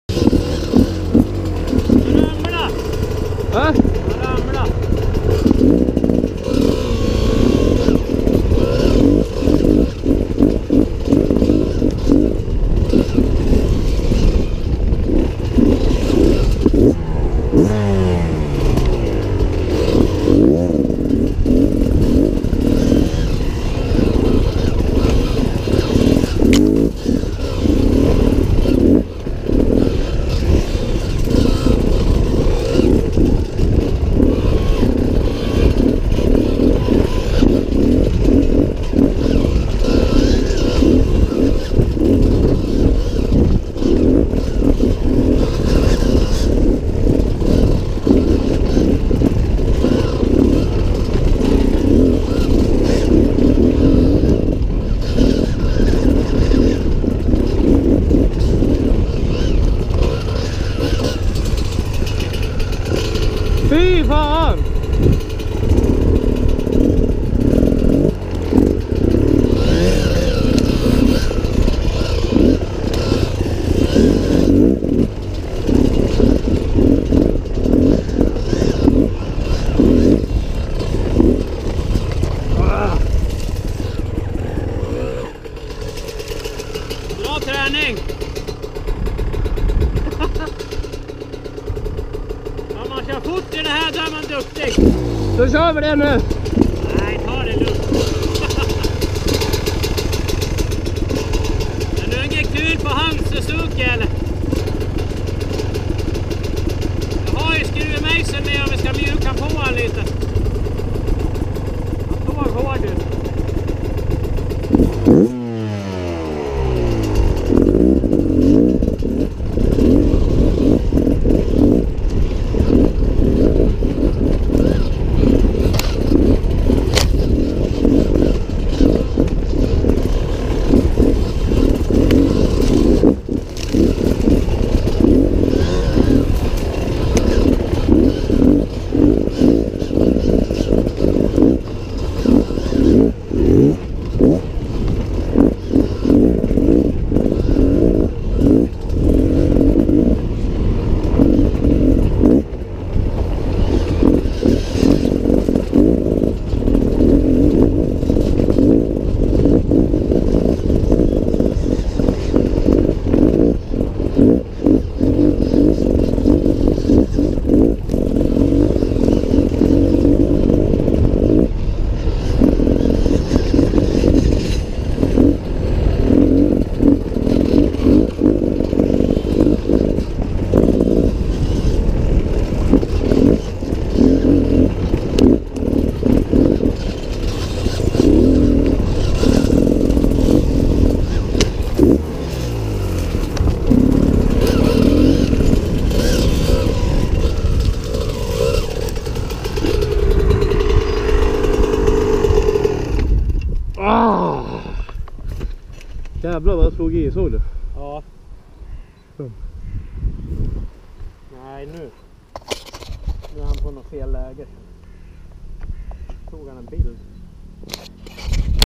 Beta xtrainer 300 Sweden Gävle
Beta xtrainer 300 in a trail full of rocks.
Stock suspension, fabrication setup.